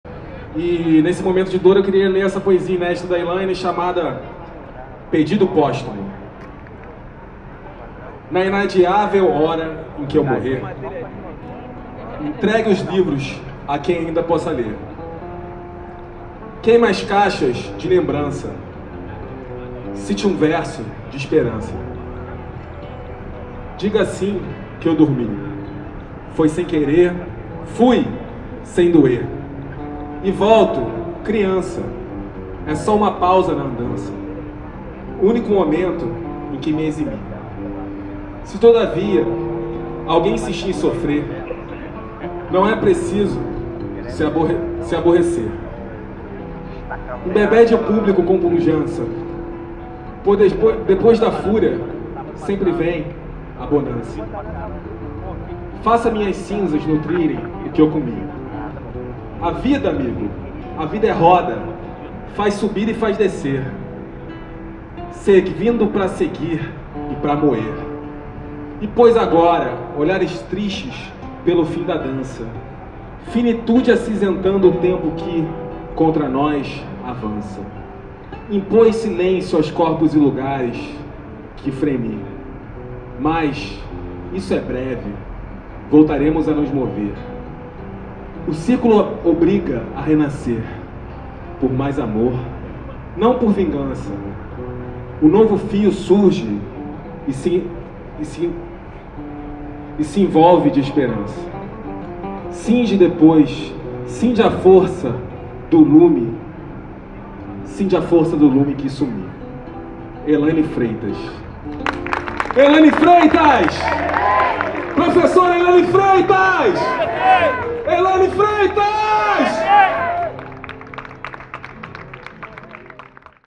Homenagem Elaine Freitas na Cinelândia
Trabalhadores da educação homenageiam a professora e poetisa Elaine Freitas em ato da educação neste dia 22 de julho na Cinelândia.